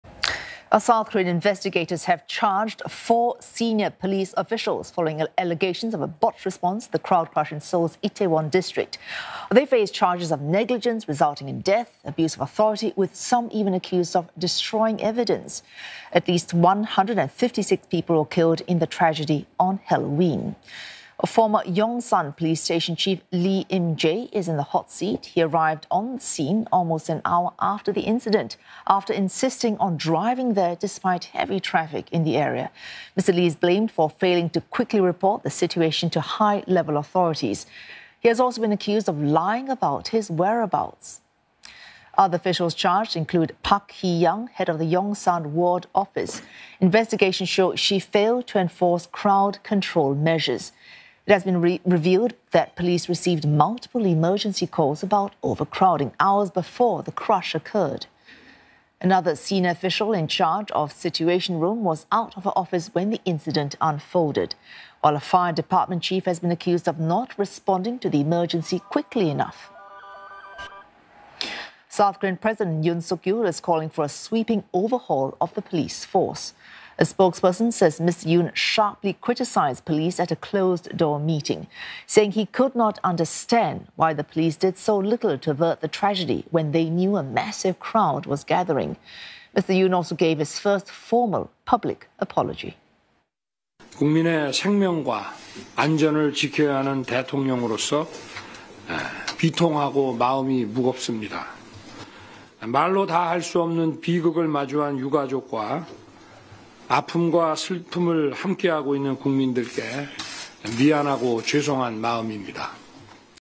Seoul Halloween crowd crush: Former Yongsan police chief, three others charged with negligence
South Korean investigators have charged four senior police officials, following allegations of a botched response to the Halloween crowd crush in Itaewon that killed at least 156. They face charges of negligence resulting in death, abuse of authority; with some even accused of destroying evidence. Former Yongsan police station chief Lee Im-jae arrived at the scene almost an hour after the incident, after insisting on driving despite heavy traffic. Mr Lee has been blamed for not quickly reporting the situation to high-level authorities.